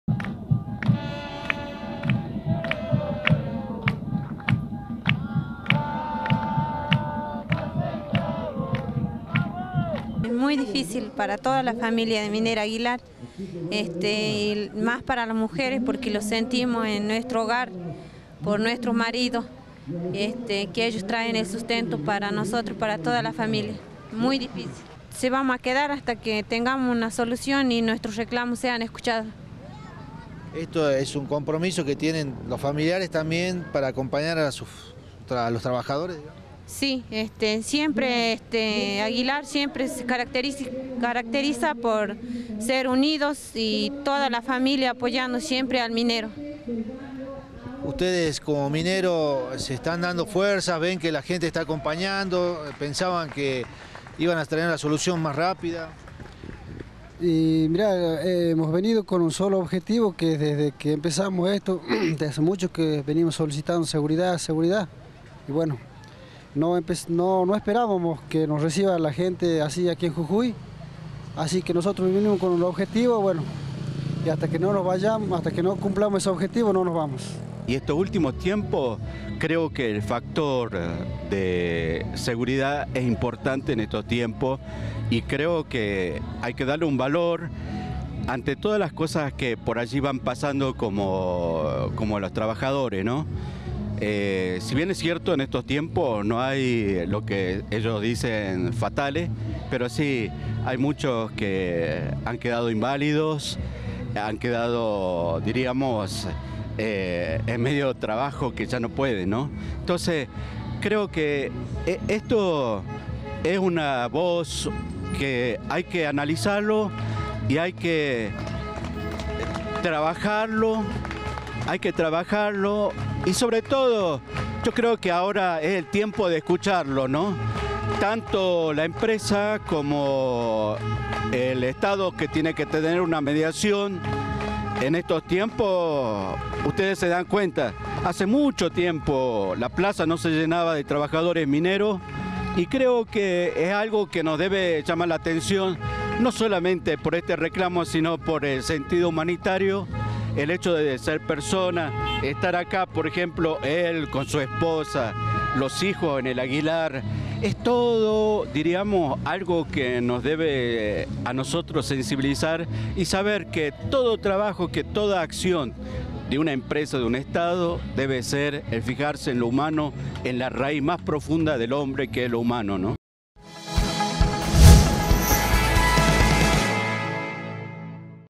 Conflicto de trabajadores de Minera Aguilar
La reunión está prevista para hoy a las 10. La empresa presentaría una propuesta superadora luego de que ayer las partes no llegaron a un acuerdo.